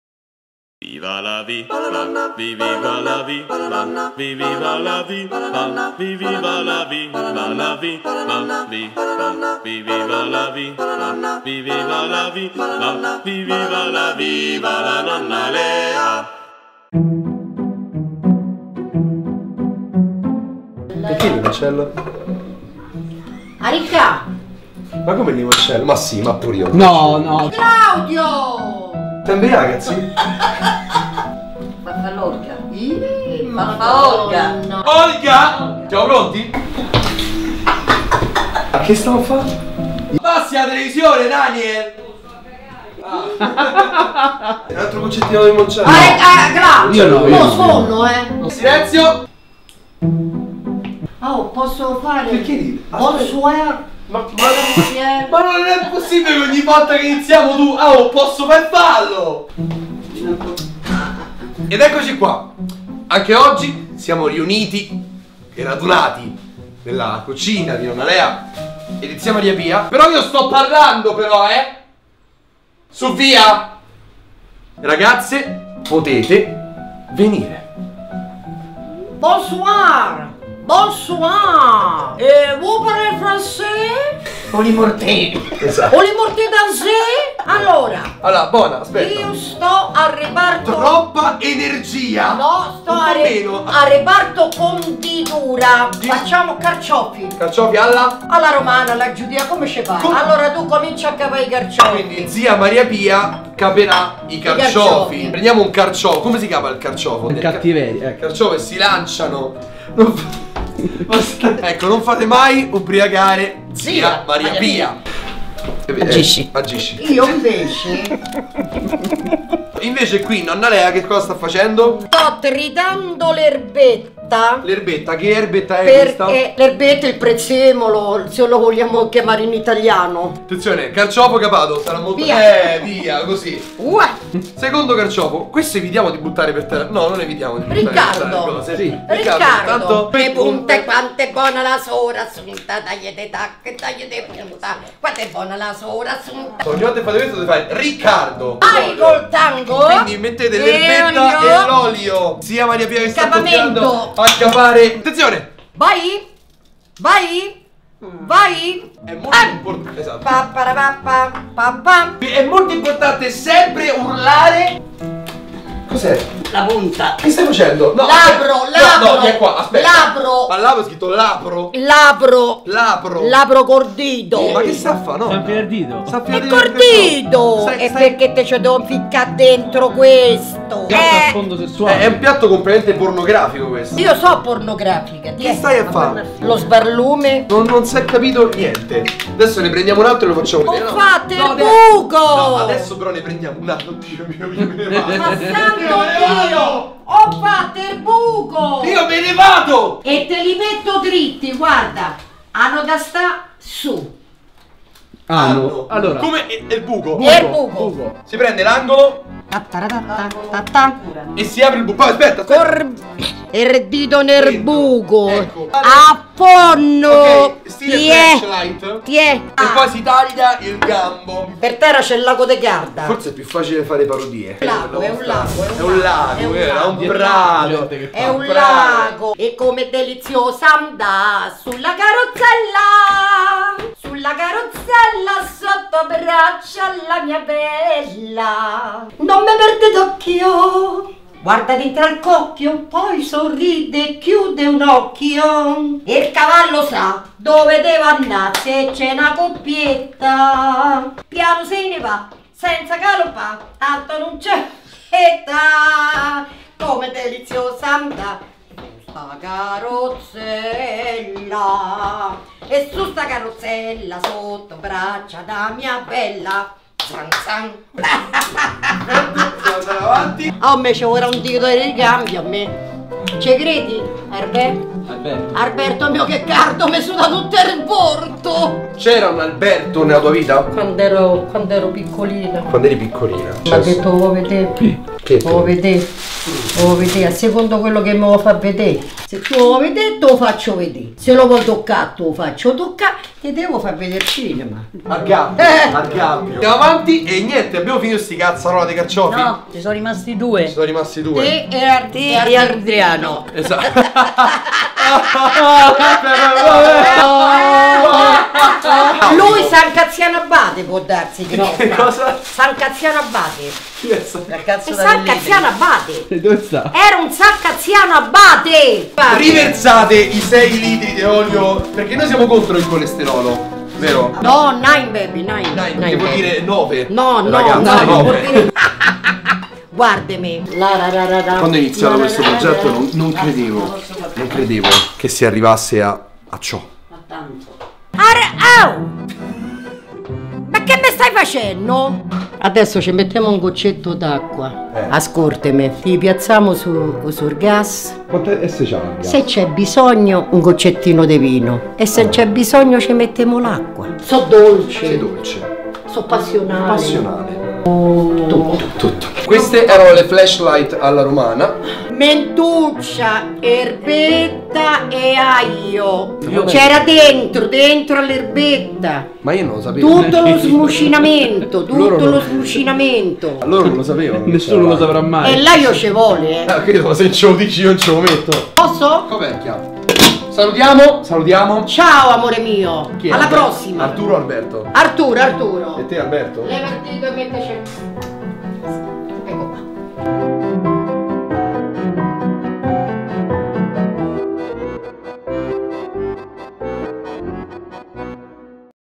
epoitelomagni 6 - I CARCIOFI ALLA ROMANA
Nonna Lea era lucida. Io e mia zia un po' di meno. Buona visione.